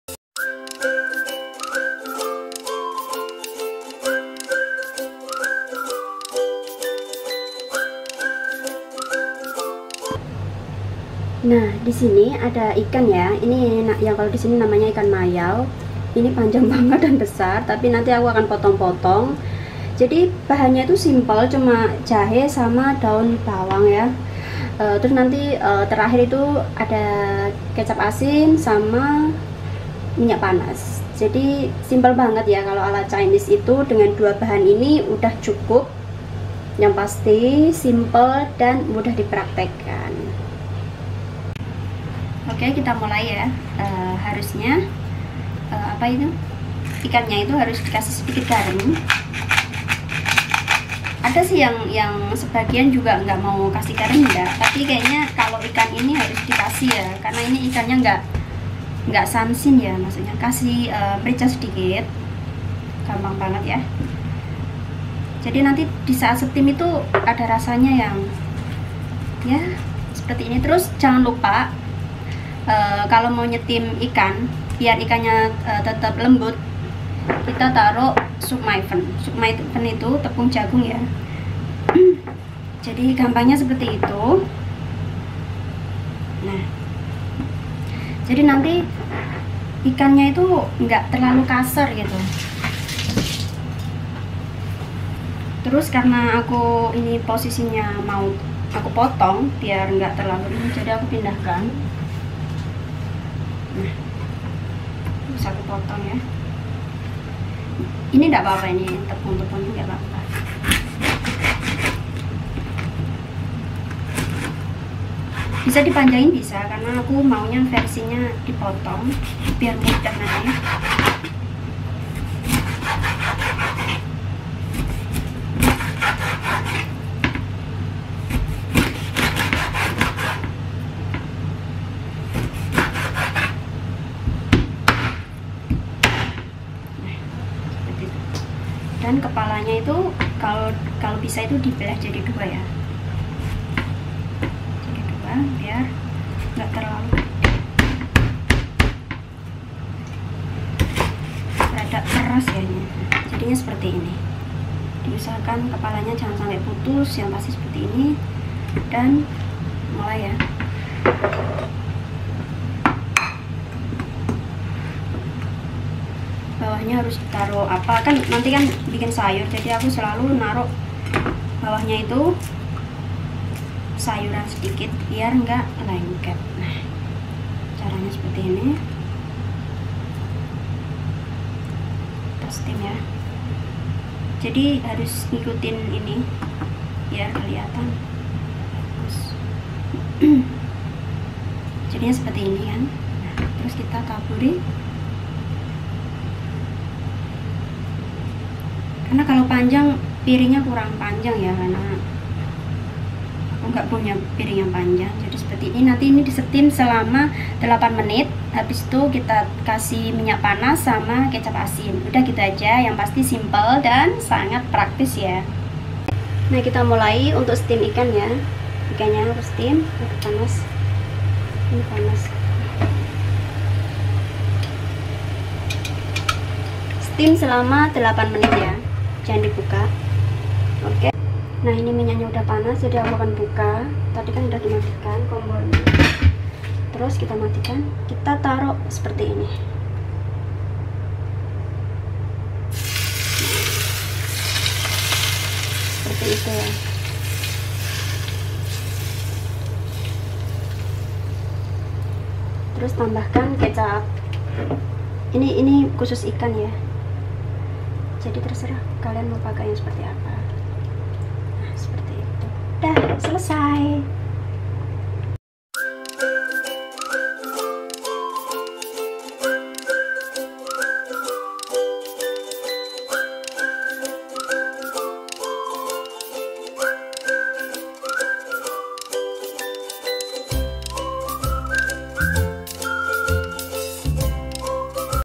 RESEP STEAM IKAN MAYAU simple style chinese dihongkong
Hallo kakakuh Kali in Aku berbagi Resep Chinese steam ikan MAYAU spt digambar vidio(Indonesia ya Gak tau APA)
Nah knp ALA Chinese it simple
Krn mrk suka yg spt HAL nya kesehatan y
Jauhi Dr yg pedes Dan yg rumit
Lihat vidio smapi usai spt APA Cara steam ikan yg simple Dan cepat dengan bahan yg sederhana...

Mksih sudah nonton
Selamat mencoba